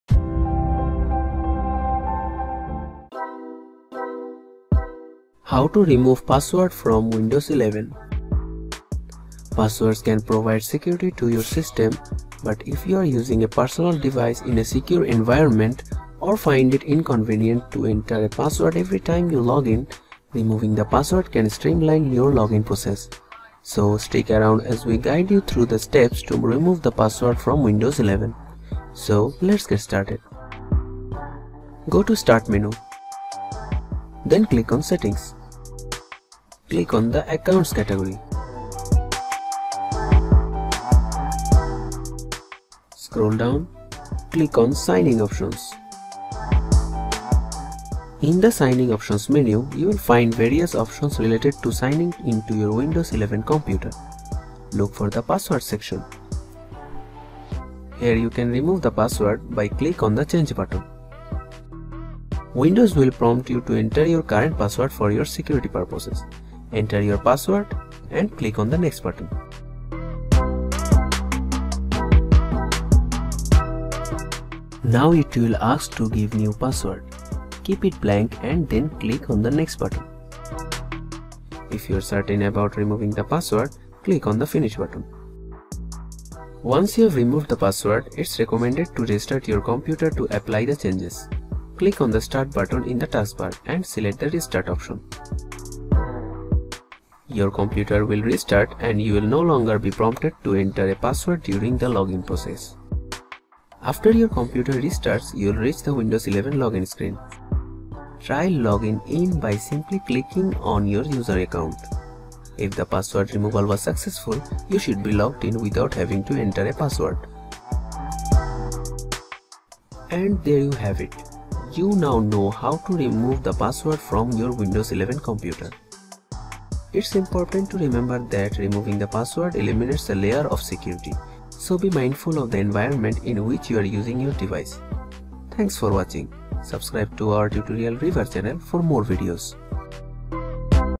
How to Remove Password from Windows 11 [ Update ] How to Disable Windows 11 Login Password
Do you want to remove your login password in Windows 11 due to some reason? Then, you can easily do that and even keep your hard drive data safe. How? Just click to know it.

Video Parts:
0:00 Info & Intro
0:32 Go to Start Menu
1:08 Enter password
1:33 Restart computer
2:11 Ending & Outro